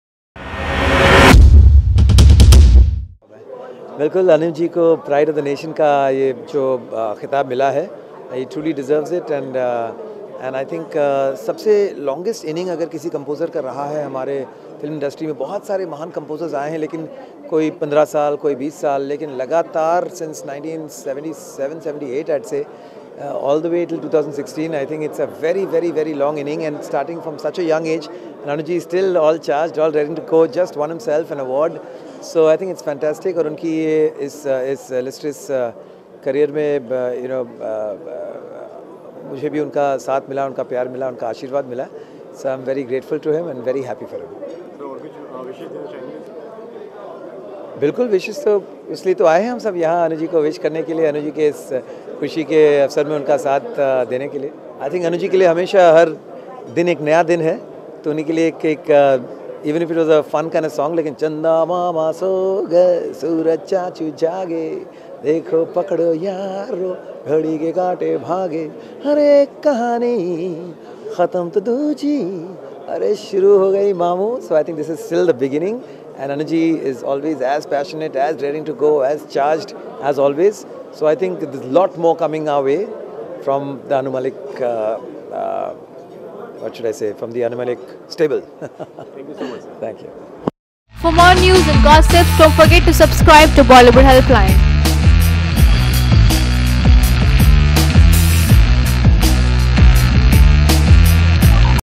Shaan At Videocon Felicitation Ceremony Of Music Director Anu Malik
Shaan At Videocon Feliciate Celebration Of Music Director Anu Malik